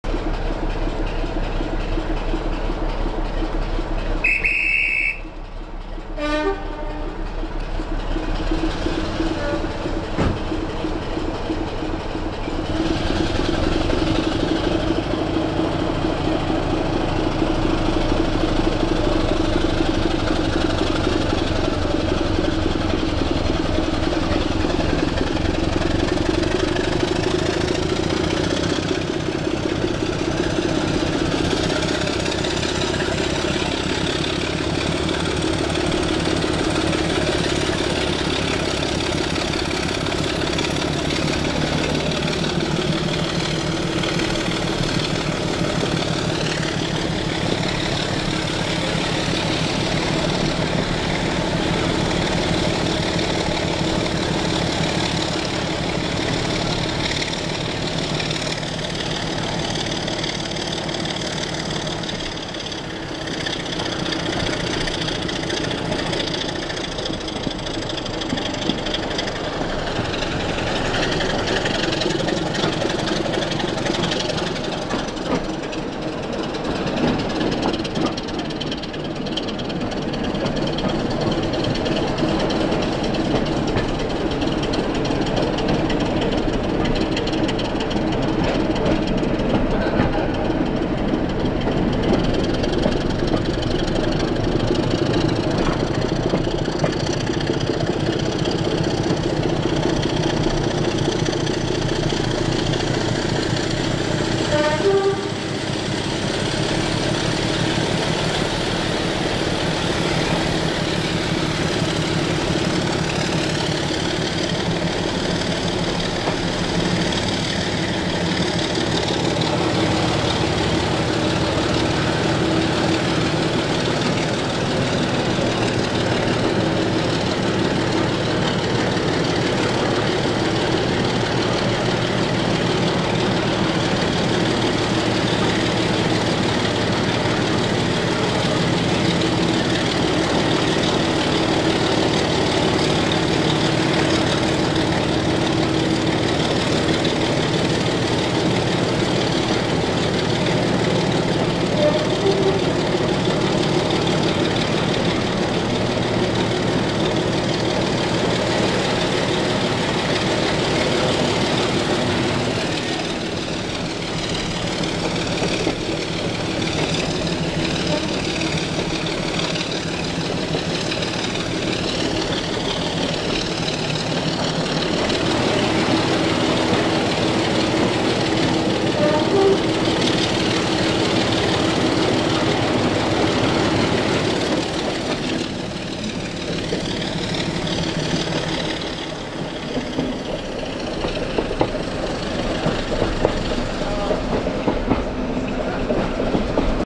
D5054 front coach Summerseat load 5 plus 45108 dead on rear 12th February 2022
D5054 is heard from the 2nd bay of the front coach leaving Summerseat. It was too rainy to poke my camera out of the window so just filmed it this way to capture the sound. 12th February 2022